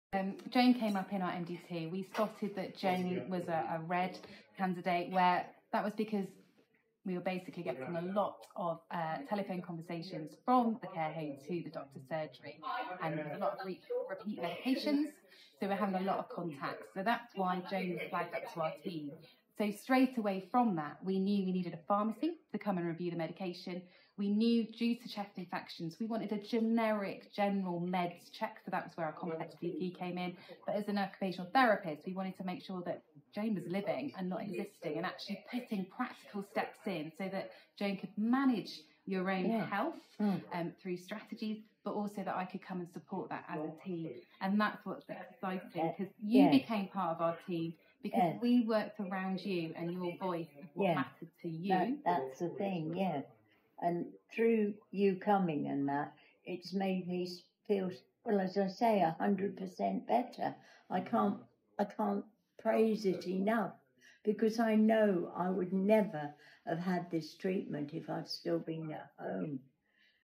Joan, and Occupational Therapist Helen talk about how BRAVE AI has helped
BRAVE AI is being used across four Primary Care Networks in Somerset.

Using clever computer algorithms, it gives each patient registered at the practice a score for how complex their health needs are and uses this to highlight those who may be at risk of their wellbeing declining to the point of needing to go to hospital. This helps health professionals identify people at risk who may otherwise go under the radar, such as patients with borderline results or who rarely contact their GP.

Here, we hear from Joan and Occupational Therapist Helen about how BRAVE AI has helped Joan's health.